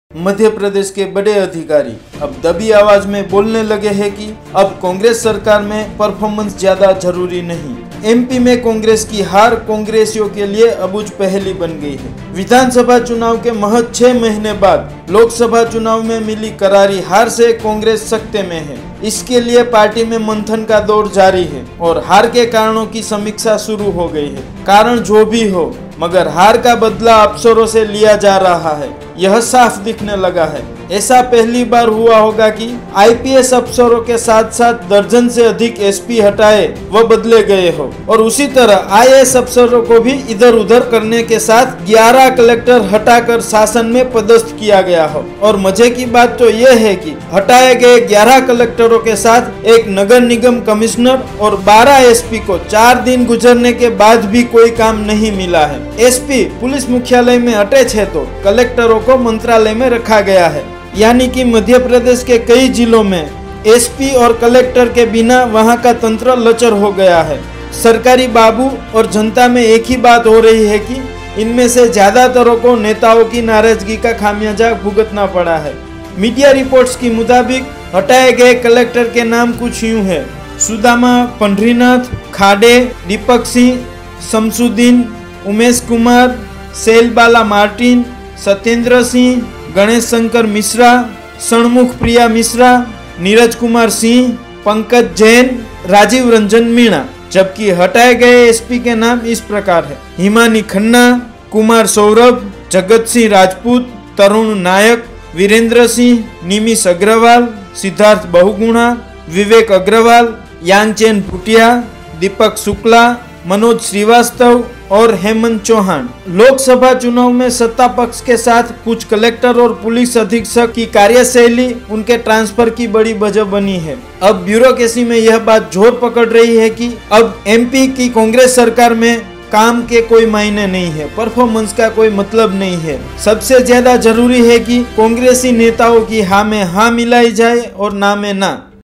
MP में हार का बदला अधिकारीओ से ?
Madhya pradesh में विधानसभा चुनाव के महज 6 माह बाद लोकसभा चुनाव में मिली करारी हार से Congress सकते में है। कारण जो भी हो मगर हार का बदला अफसरों से लिया जा रहा है यह साफ दिखने लगा है, IPS अफसरों के साथ साथ  दर्जन से अधिक SP हटाने व बदले गए हो  और इसी तरह IAS अफसरों को इधर-उधर करने के साथ 11 कलेक्टर हटाकर शासन में पदस्थ किया गया हो हटाए गए 11 कलेक्टरों के साथ एक नगर निगम कमिश्नर और 12 एसपी को चार दिन गुजरने के बाद भी कोई काम नहीं मिला है।